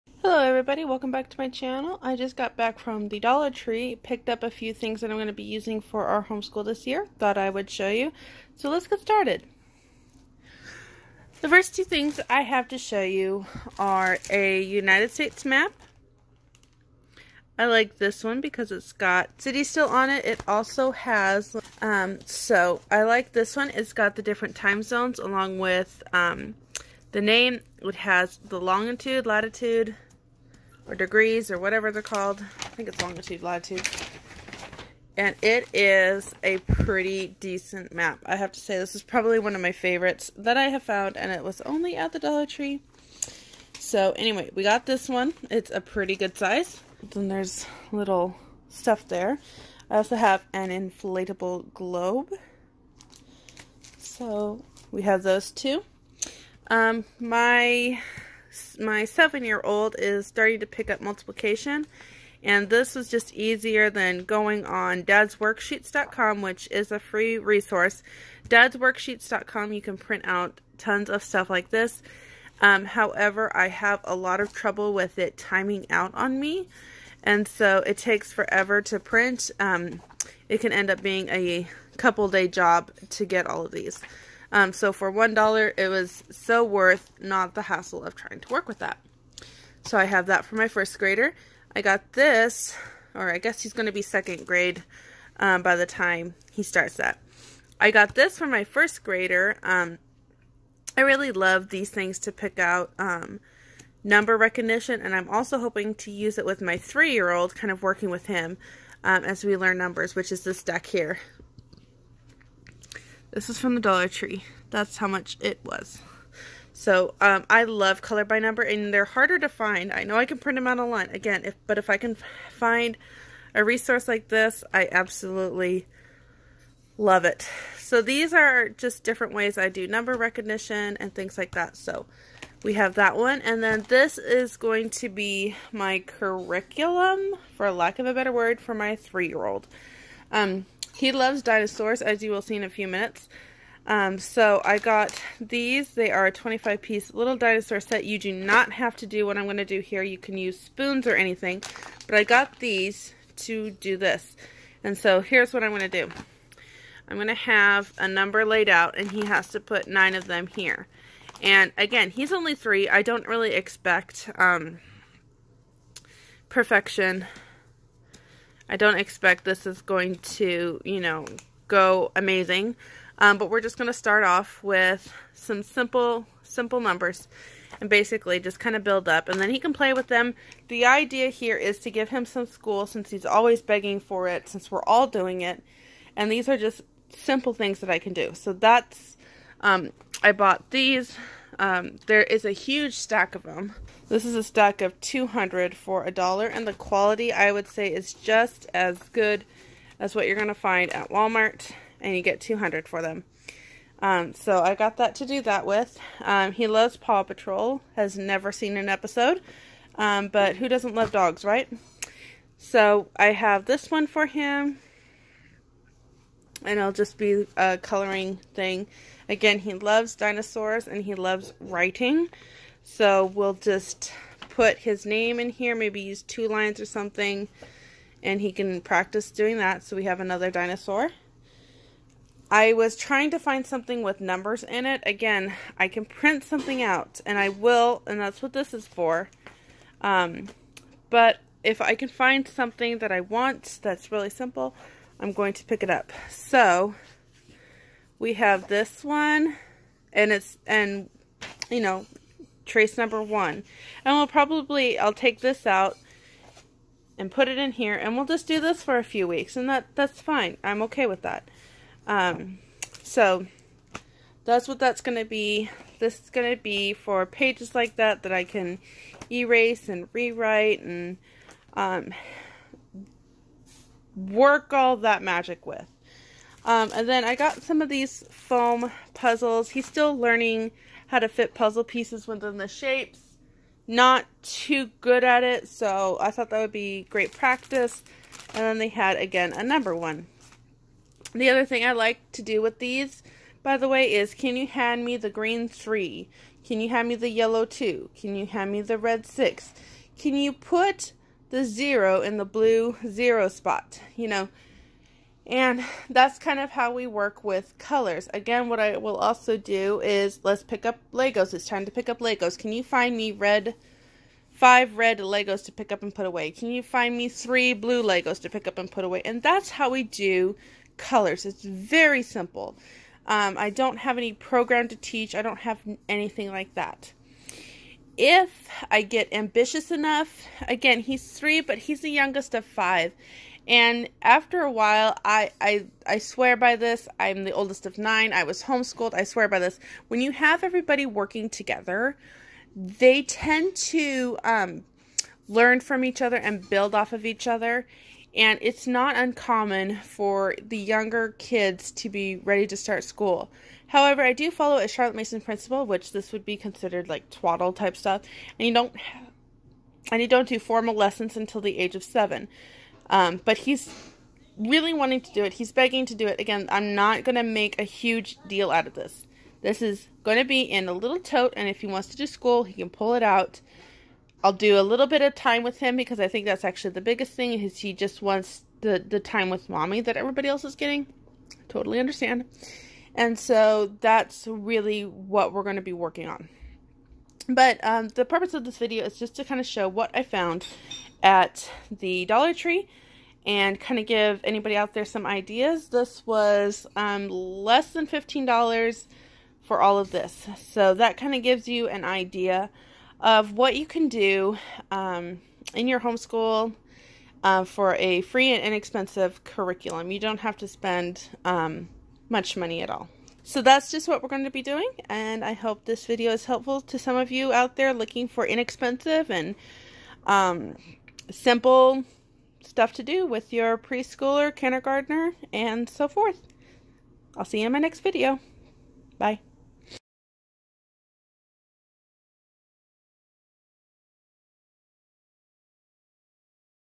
Affordable and Minimal Preschool Curriculum
Hope you enjoy this video and the ideas. It's a Dollar Tree haul but also how I put together my youngest's curriculum.

Our Dollar Tree had some pretty neat stuff that I was able to piece together a "school box" for my youngest student, who just turned three. He has four older siblings as well that he picks things up from and learns and wanted his own school. Super simple. Keep it low key and let them learn at their pace!